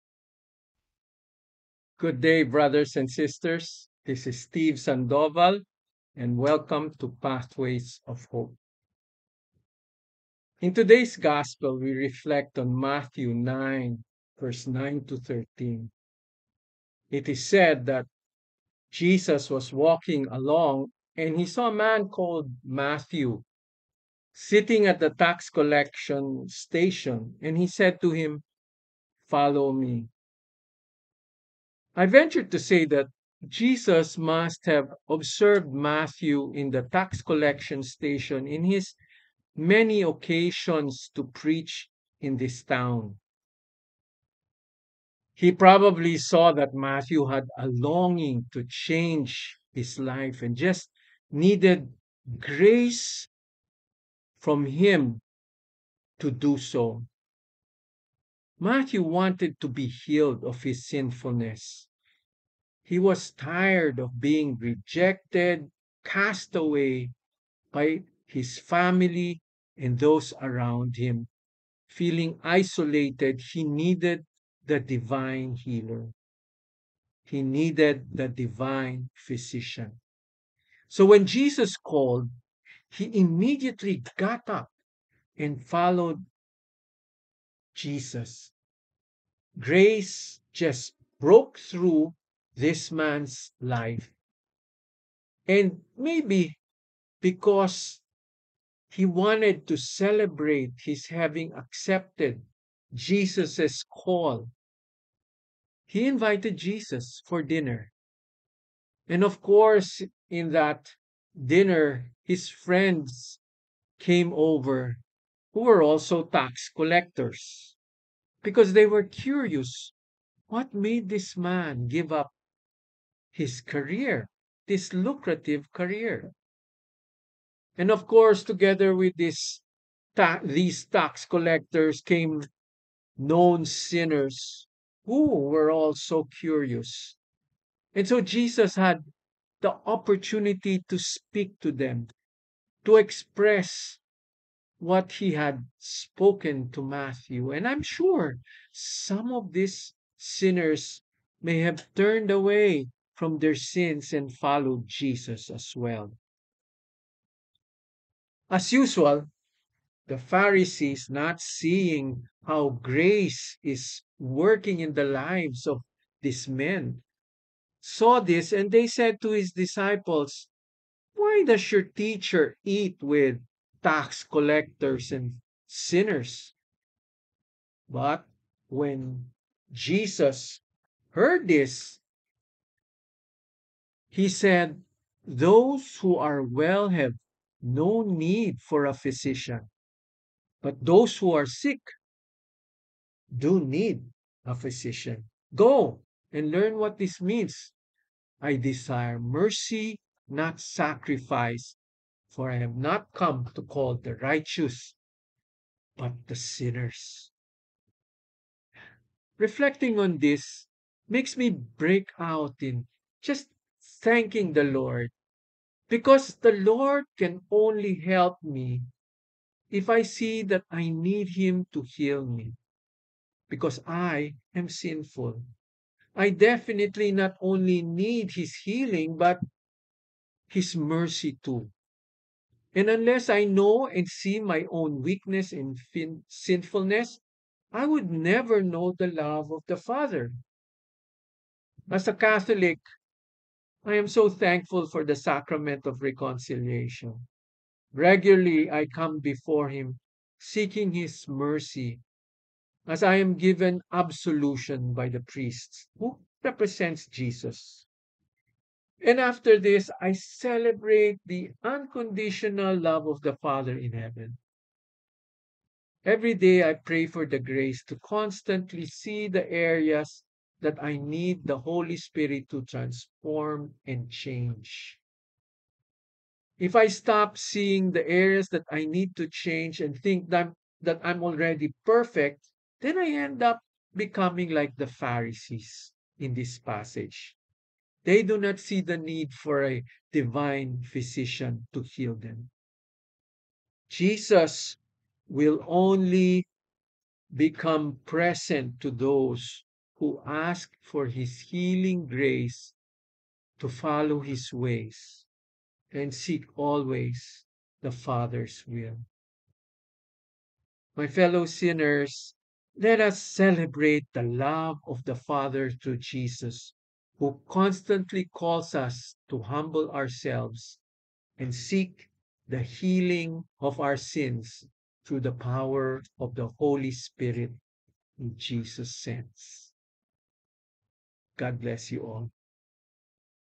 PATHWAYS OF HOPE : “Lessons in Grace and Compassion”
Thursday of the 24th Week in Ordinary Time
Feast of Saint Matthew, Apostle and Evangelist

[GOSPEL OF THE DAY] :  MATTHEW 9: 9 - 13

As Jesus passed by,
he saw a man named Matthew sitting at the customs post.
And he got up and followed him.
While he was at table in his house,
many tax collectors and sinners came
and sat with Jesus and his disciples.
The Pharisees saw this and said to his disciples,
"Why does your teacher eat with tax collectors and sinners?"
He heard this and said,
"Those who are well do not need a physician, but the sick do.
Go and learn the meaning of the words,
I desire mercy, not sacrifice.
I did not come to call the righteous but sinners."

Video by teve Sandoval : CEO and Training Director-LeadCore Training and Consultancy. Strategic Planner;  Organizational Development Expert;  Pastoral Leader-Ang Ligaya ng Panginoon;  Head of Training and Development -Pathways Ministry